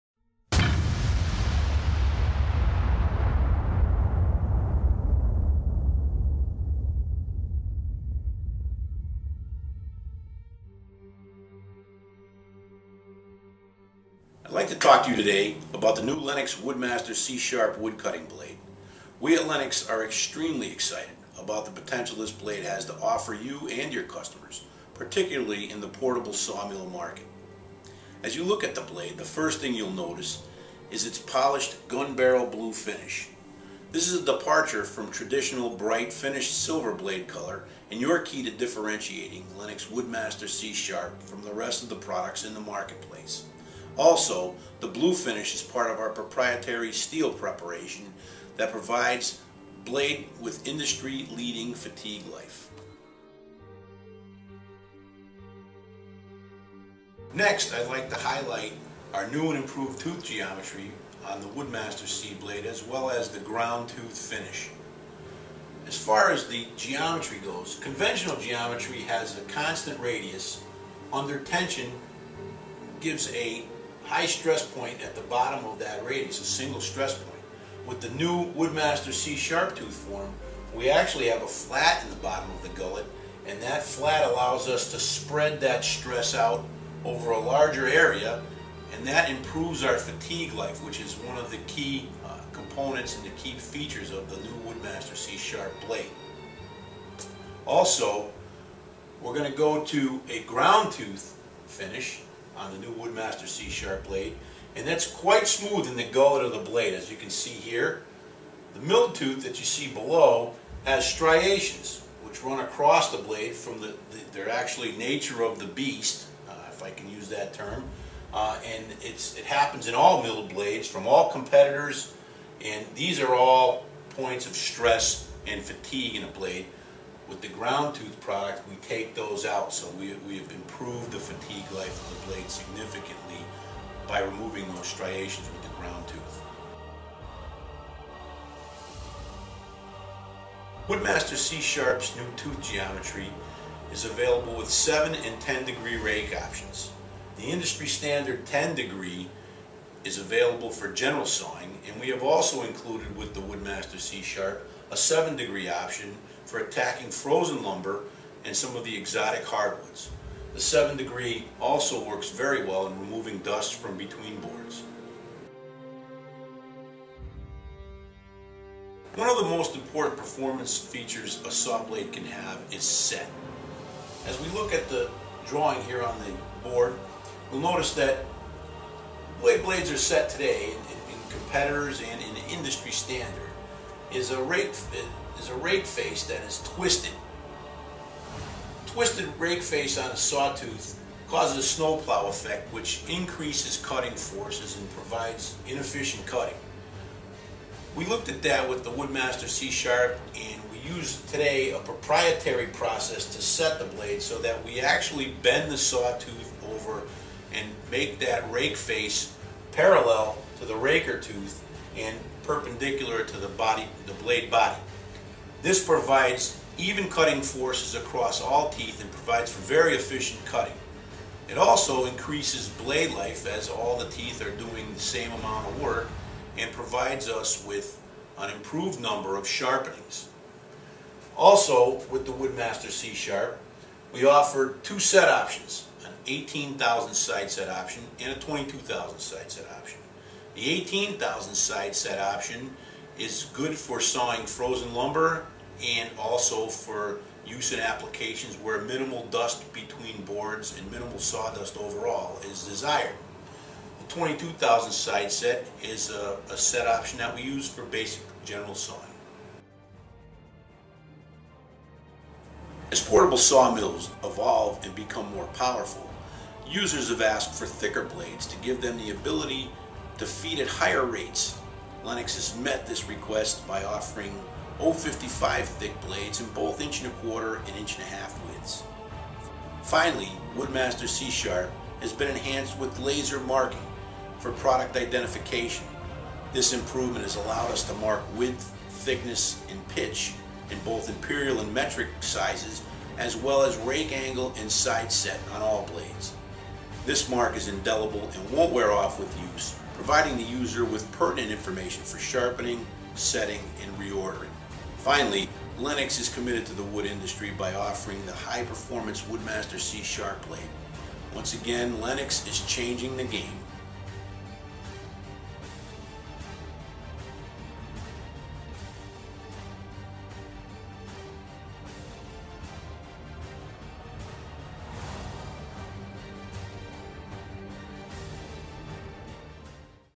WOODMASTER C SHARP FEATURES & BENEFITS
LENOX engineer Joe Maiolo explains the features & benefits of the newest band saw blade for wood cutting from LENOX - WOODMASTER C-SHARP.